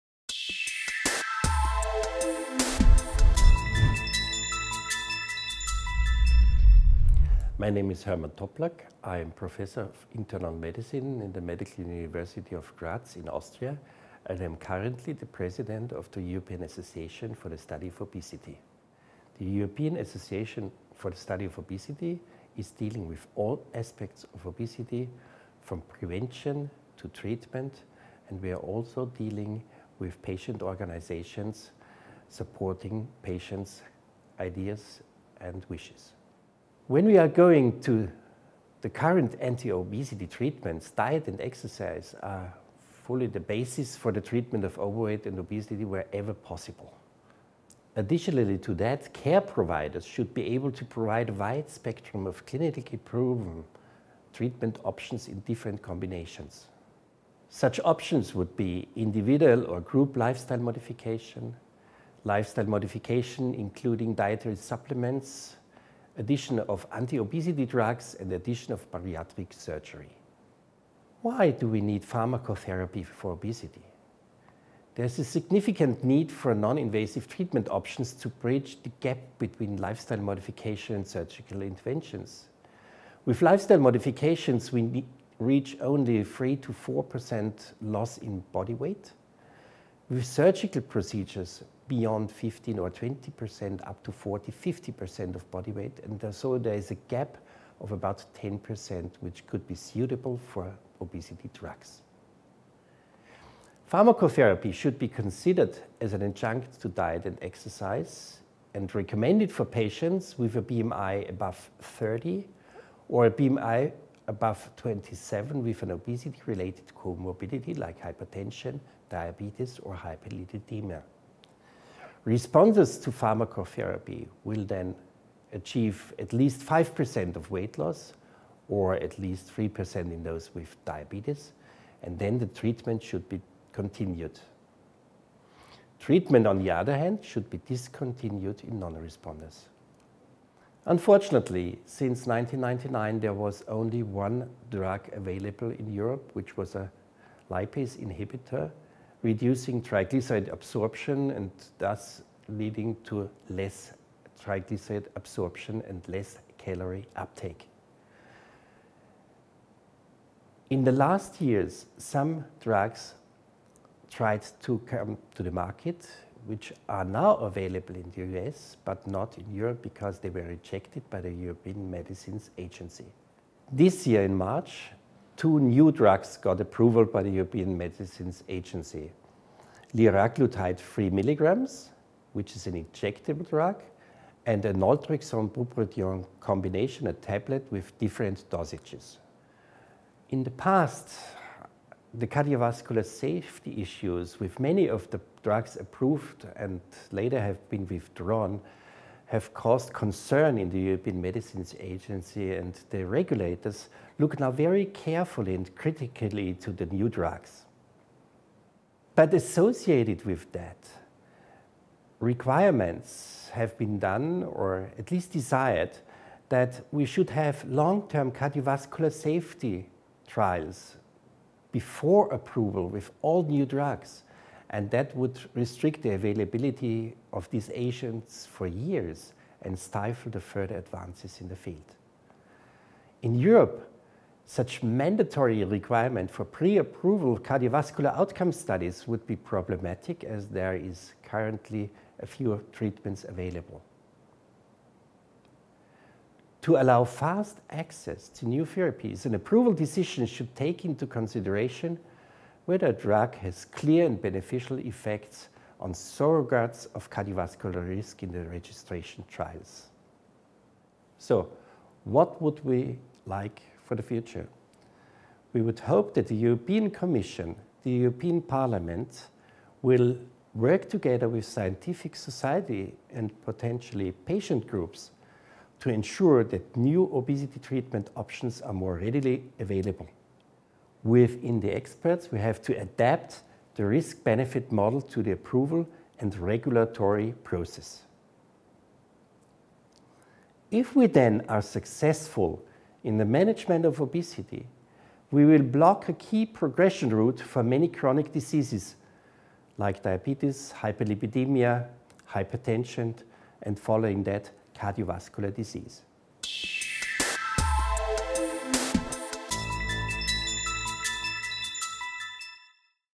The need for anti-obesity drugs in obesity management
Professor Hermann Toplak
EASO President
Associate Professor, Internal Medicine, Medical University Graz, Austria